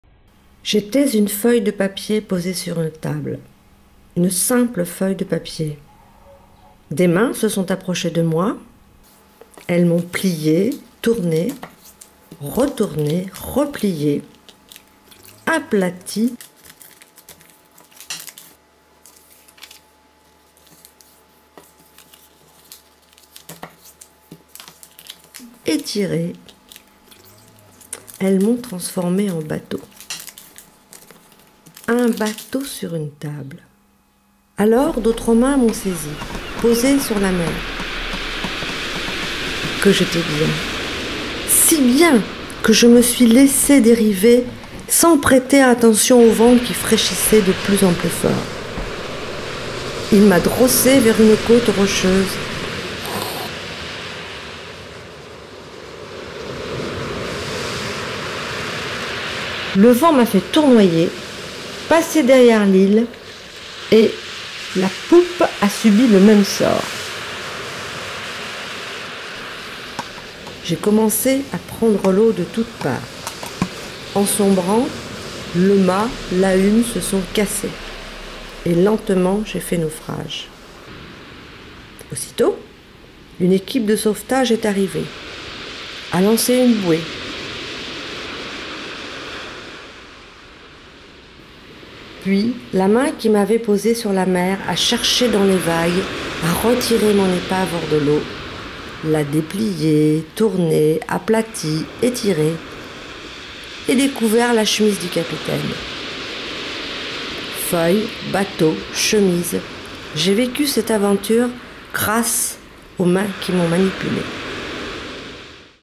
feuille bateau .avi
exercice de cours vidéo,  imaginez un" sénario" et construire un petit film en une seule prise, cela tombe bien,  ce jour là je porte une veste bleue, vite une idée, pliage, découpage, c'est parti.